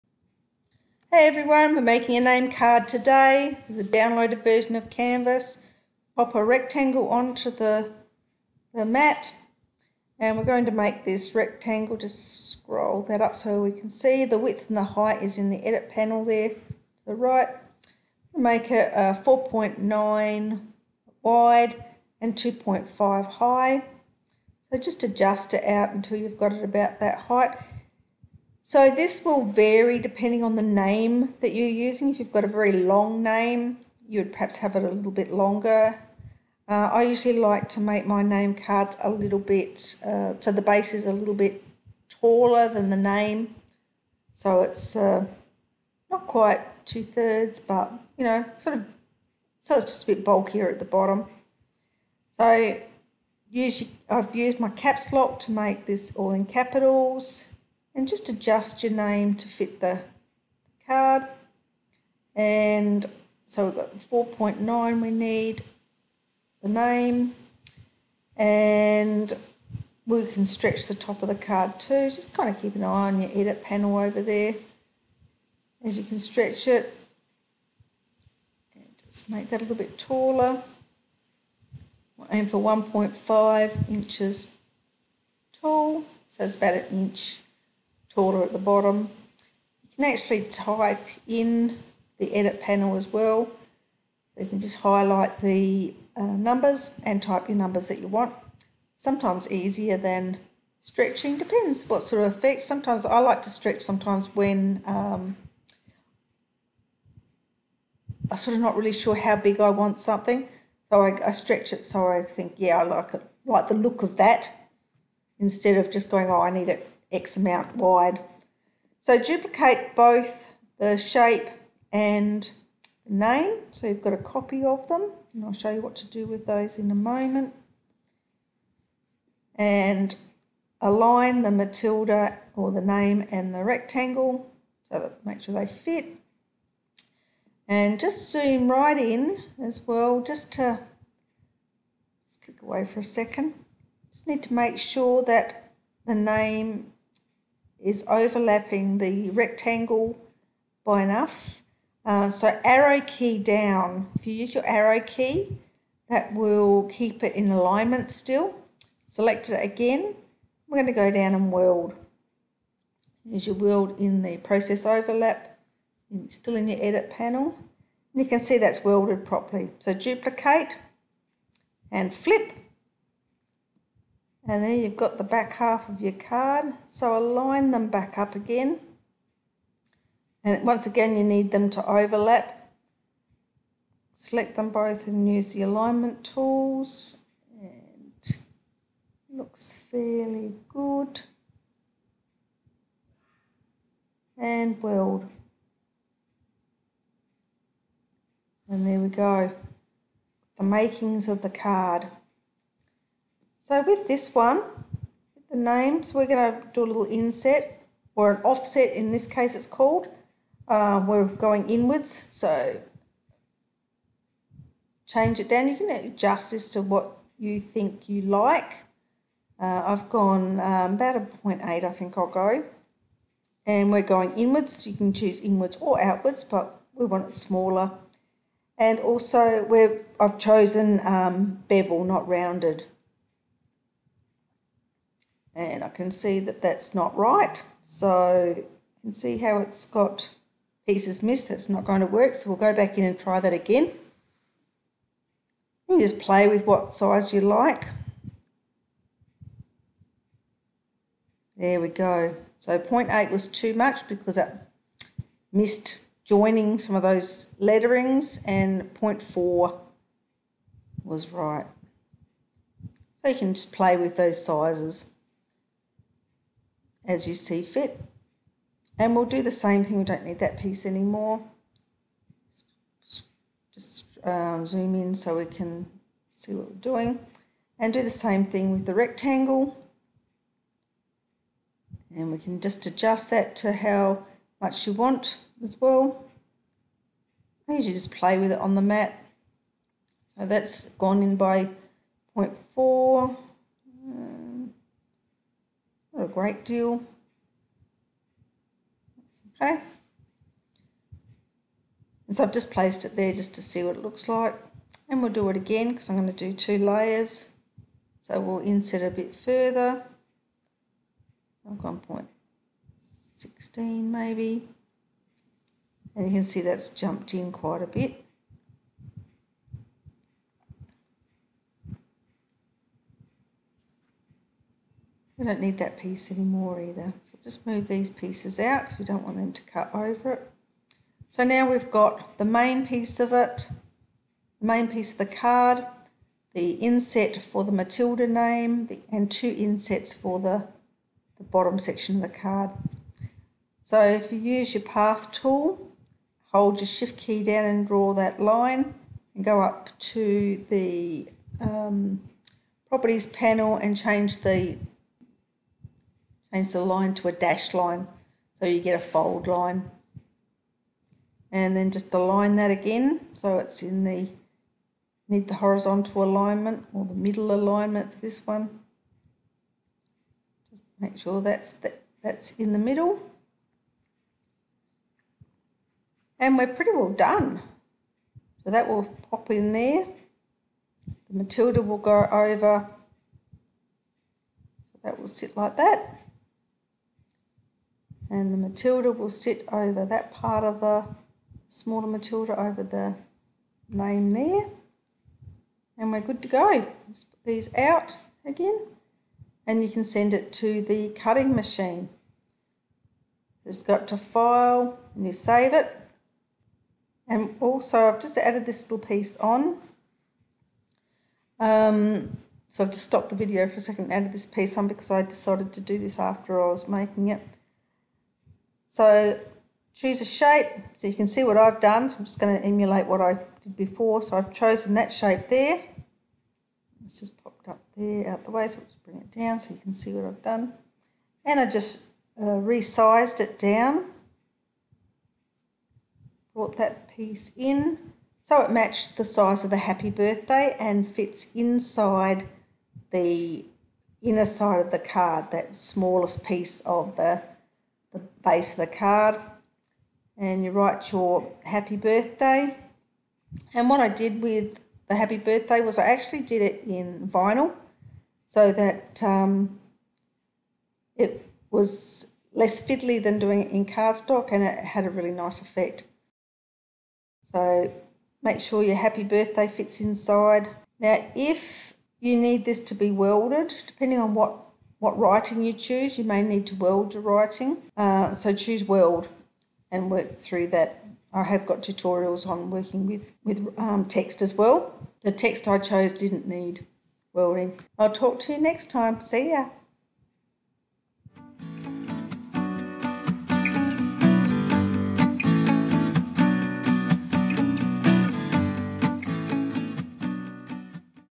Canvas Workspace | How to Make a Name Card | Scan N Cut Tutorial
In this tutorial, I’ll show you how to create a personalized name card using the offline/downloaded version of Canvas Workspace, perfect for cutting on your Scan N Cut machine! 🎨✂️

This project is fun and easy to follow, allowing you to design a unique name card that’s fully customizable for anyone. Whether it’s for birthdays, special occasions, or just a fun way to personalize your crafts, this is a great way to make your cards extra special.

📌 In this video, you’ll learn:

* How to design a name card in Canvas Workspace
* Steps to prepare your design for cutting on your Scan N Cut
* Tips for getting the perfect cut every time!

Grab your machine, and let's get started on this creative project! ✨

Did you know that there are two versions of Canvas Workspace?

Want to learn how to use Canvas Workspace (download version)? This is the one I love and recommend.
You will find the Get Started With Canvas Workspace course

To enroll in the FREE Scan N Cut Basics course go

BROTHER ACCESSORIES